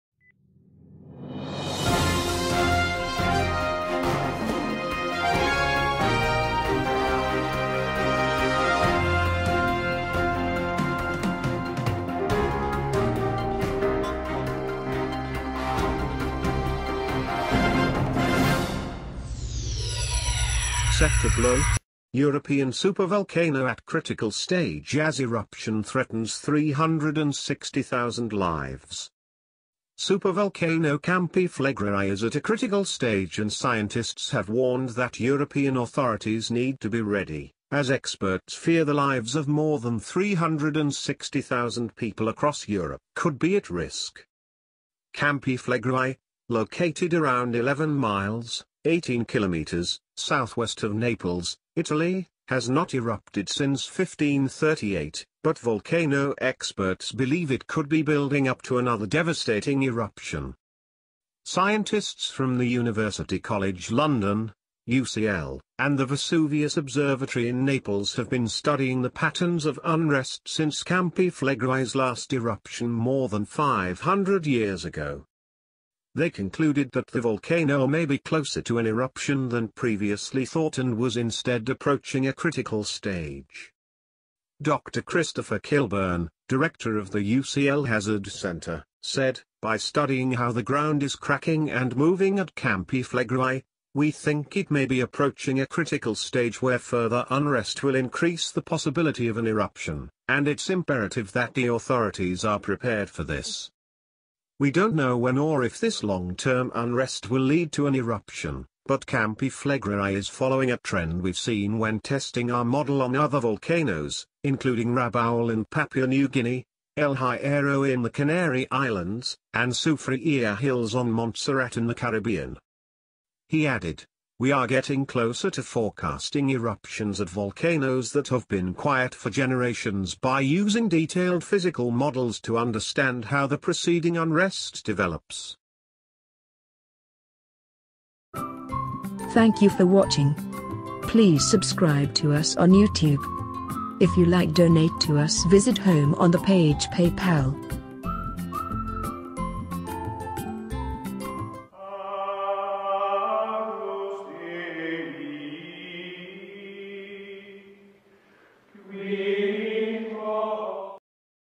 Ready to blow European supervolcano at 'CRITICAL STAGE
Set to blow? European supervolcano at 'CRITICAL STAGE' as eruption threatens 360,000 lives

SUPERVOLCANO Campi Flegrei is at a critical stage and scientists have warned that European authorities need to be ready – as experts fear the lives of more than 360,000 people across Europe could be at risk.

Campi Flegrei, located around 11 miles (18 kilometres) southwest of Naples, Italy, has not erupted since 1538, but volcano experts believe it could be building up to another devastating eruption.

Scientists from the University College London (UCL) and the Vesuvius Observatory in Naples have been studying the patterns of unrest since Campi Flegrei's last eruption more than 500 years ago.